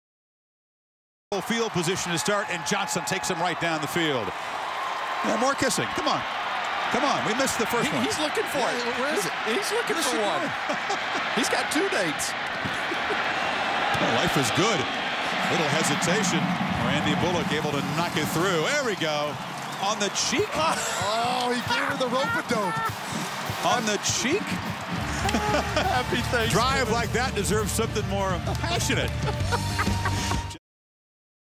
Texas-Texas A&M - Aggie kiss fail
This Texas A&M cadet just wanted a kiss from his girl, but there were some problems. From the Texas-Texas A&M game on November 26, 2009.